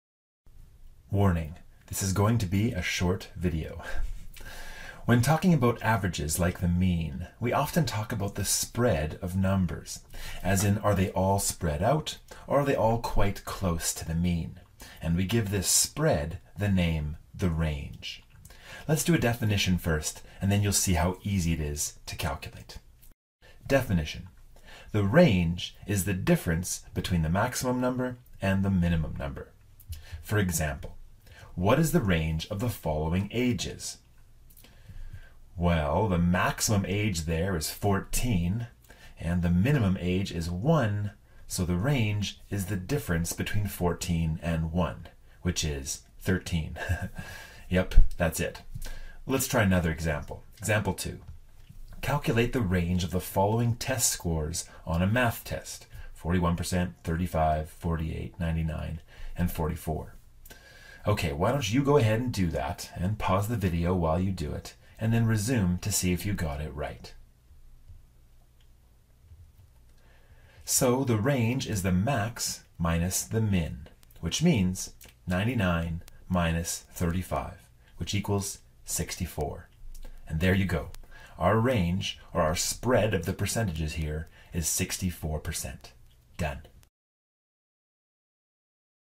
WCLN - Data Analysis Part 5: Range
The range of a data set measures how far apart the numbers are.